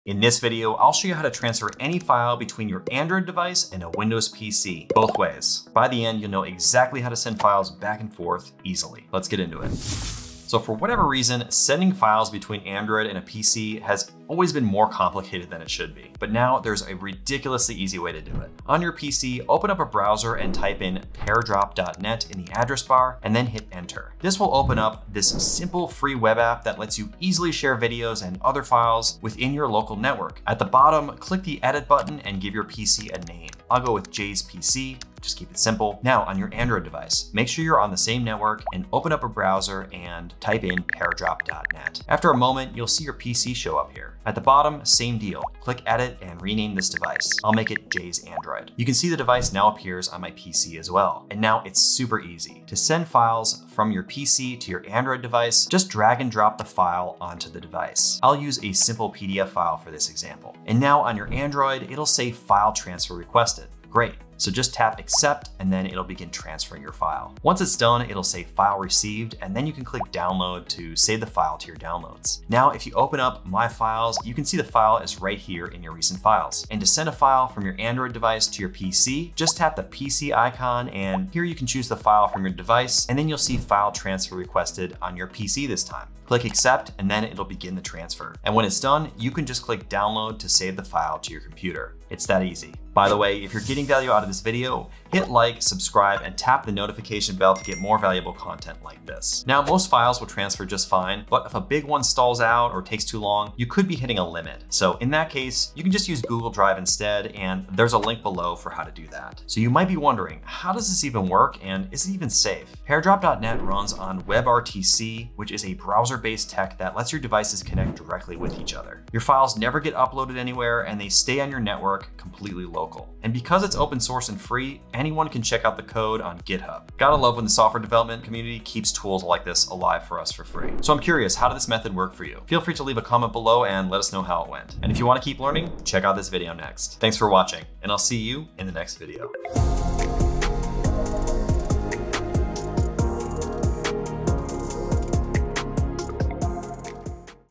How To Transfer ANY FILE from Android to PC (Both Ways)
You'll learn how to transfer ANY FILE between Android and Windows PC, both ways. By the end, you'll be able to send photos, videos, and other files in either direction, quickly and easily.

Chapters:
0:00 Intro
0:10 Overview
0:20 Set up Windows PC
0:36 Set up Android device
0:51 Send files from PC to Android
1:16 Send files from Android to PC
1:30 Subscribe
1:40 Limits and Google Drive for large files
1:50 How it works, safety, and security
2:11 Outro